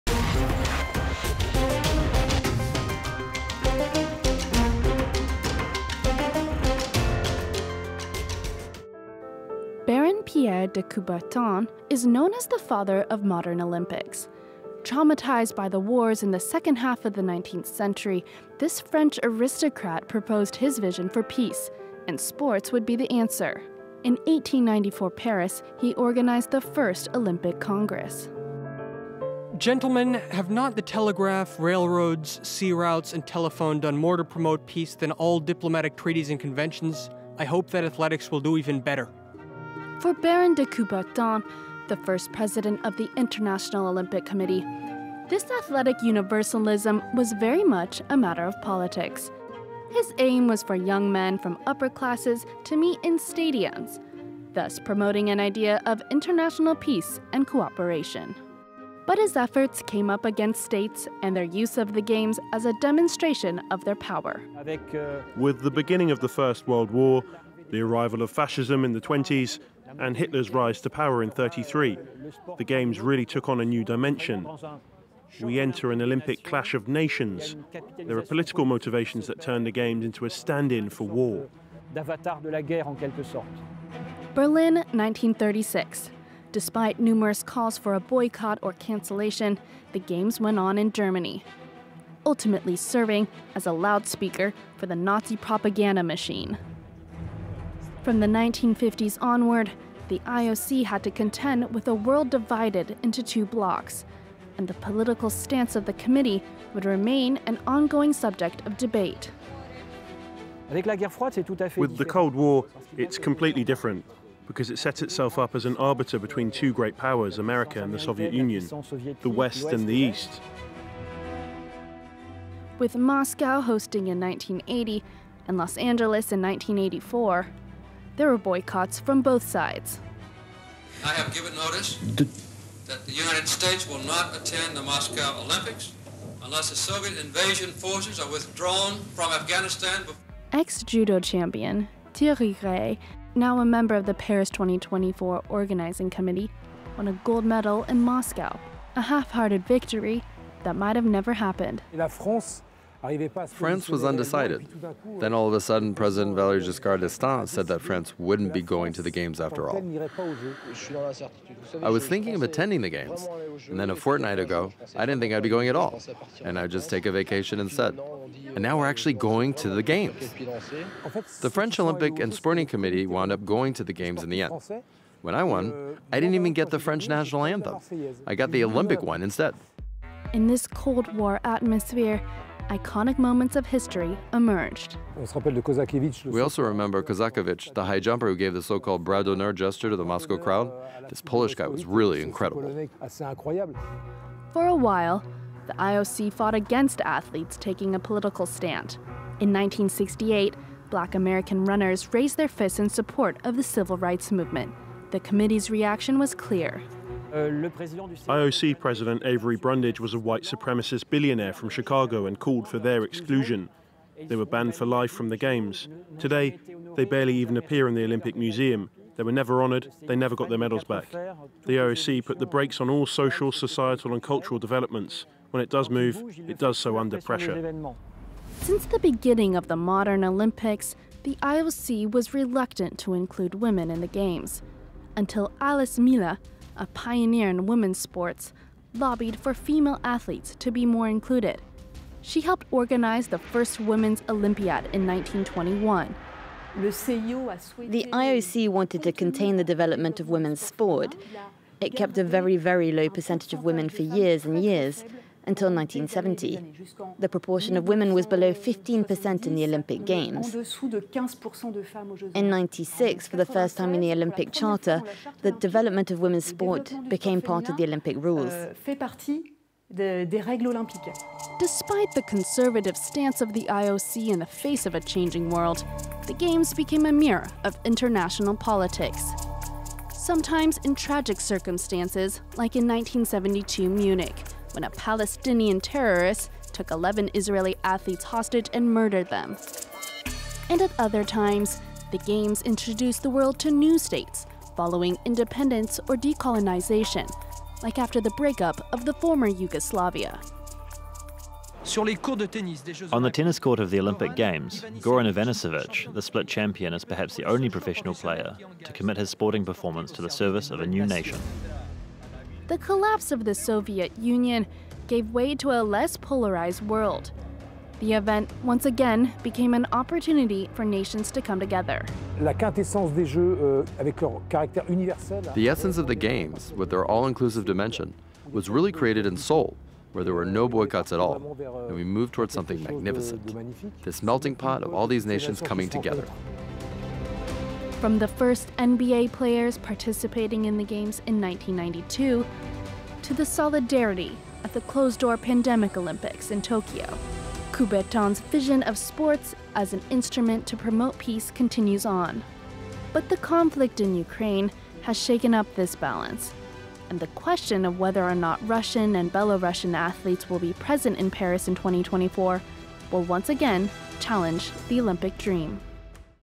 Geopolitics and the Olympics: When the IOC struggles to stay on the sidelines • FRANCE 24 English
When French aristocrat Pierre de Coubertin revived the Olympics in 1894, he imagined the Games as an occasion for world peace as countries came together in amicable sporting events. But the Games were soon caught up in nations' rivalries and political agendas. The International Olympic Committee (IOC) struggled – and failed – to distance itself from the 20th century's global crises and conflicts, from holding the Berlin Olympics in 1936 during Nazi rule to rival boycotts of the Games during the Cold War. FRANCE 24's David Gilberg and Lauren Bain take a look back.